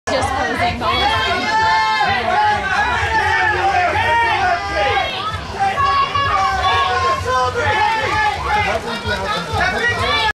Kate Hudson at amfAR's Inspiration Gala Los Angeles on 8/...
Kate Hudson at amfAR's Inspiration Gala Los Angeles on 8/11/12 in Los Angeles, CA